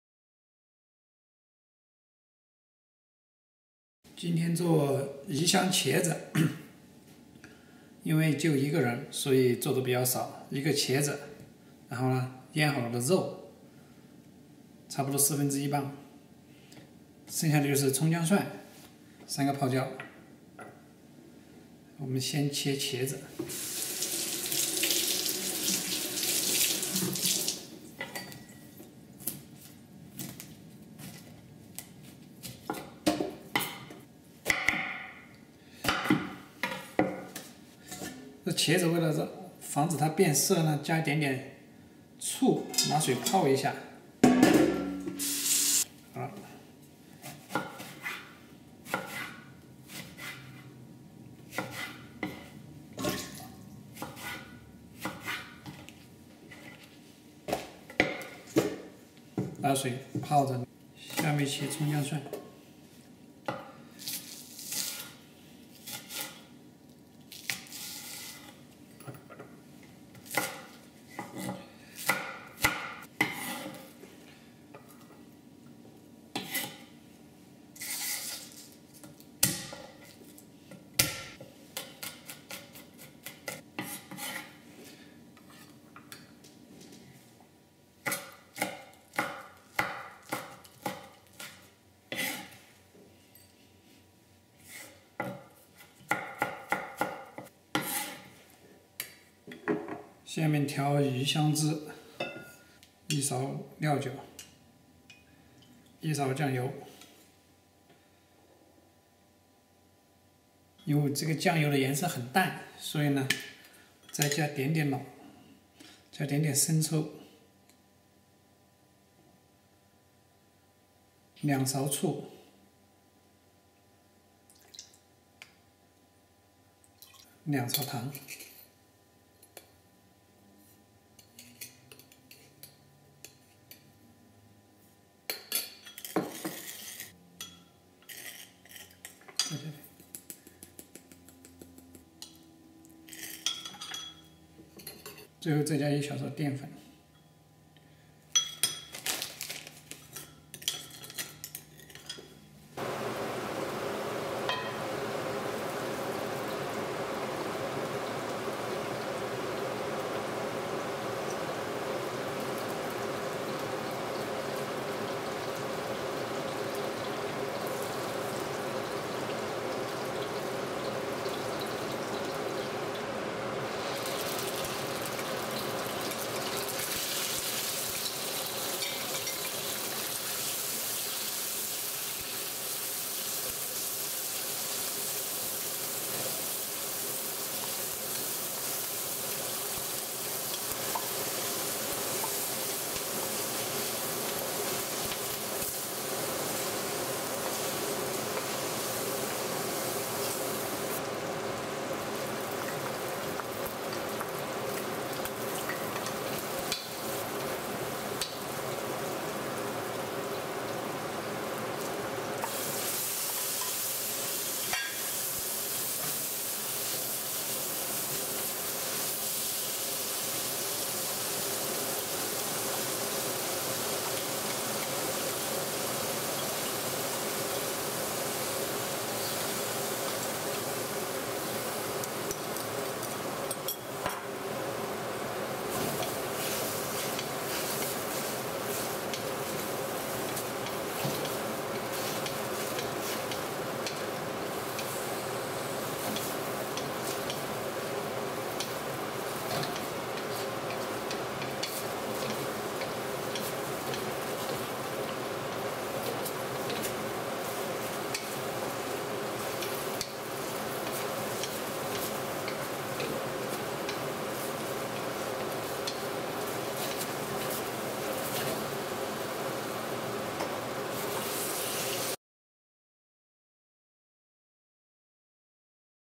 鱼香茄子(Eggplan w/ Hot Garlic Sauce)
鱼香茄子, 甜酸可口。肉可以少放一点， 也可以不放。腌肉的过程可以参见我以前的视频。

Eggplant w/ Hot Garlic Sauce, sour and sweet. Can reduce the amount of pork, or no pork. See my previous videos for marinating pork.

Ingredient:
Pork: 1/4 lbs
Chinese Eggplant: 1 large
Garlic: 2 clover
Scallion: 2
Ginger: 2 pieces
Pickled Chili: 3
Vegetable Oil: 4 tablespoon

Marinate Pork with:
Cooking Wine: 1 tablespoon
Soy Sauce: 1 tablespoon
Salt: 1/2 teaspoon
White Pepper: 1/2 teaspoon
Vegetable Oil: 1 teaspoon

Sauce:
Vinegar: 2 tablespoon
Sugar: 2 tablespoon
Salt: 1 teaspoon
Starch: 1 teaspoon